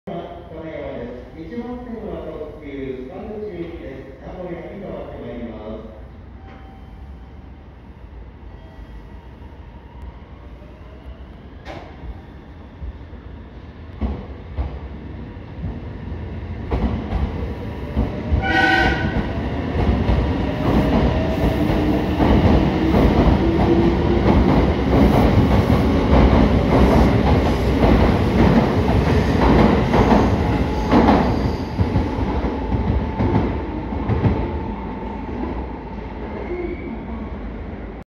●20210115 3300×9100系 発車 ＠金山
撮影路線：名鉄 名古屋本線
撮影駅：金山
行先：準急 中部国際空港
車両：3300×9100系
両数：4+2両＝6両
MH：ーー
備考欄：